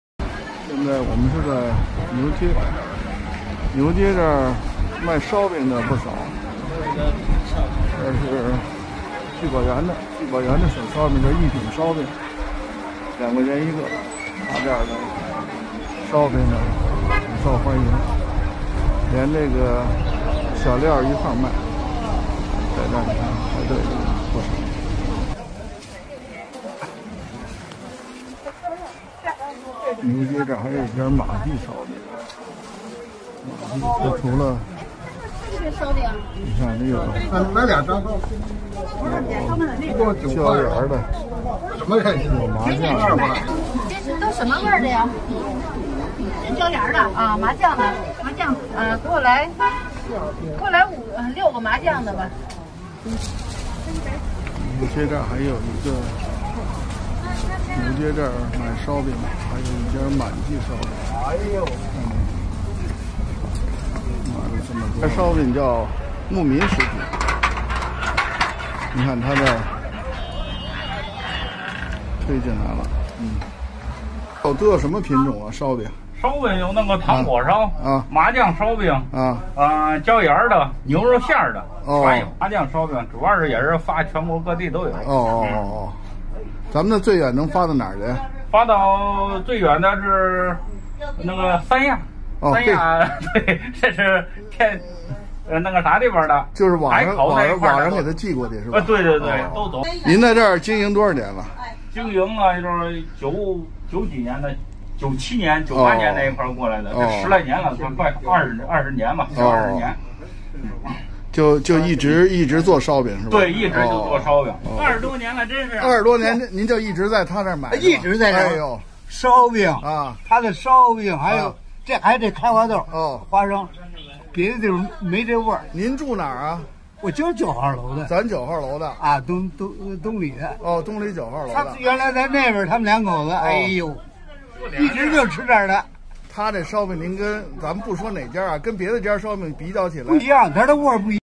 【北京街头】逛牛街：这四家口碑好的烧饼店，您得这一口儿
土生土长的老北京带您逛北京